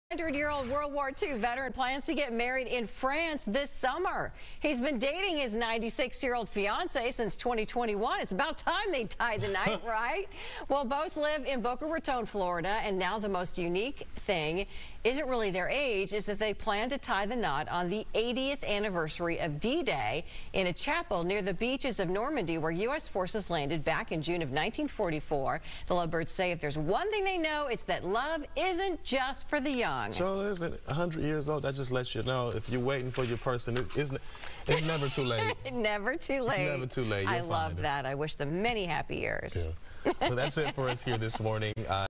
100-year-old WWII Vet to marry on 80th Anniversary of D-Day
A 100-year-old vet is prepared to marry his 91-year-old fiancé in France on the anniversary of D-Day.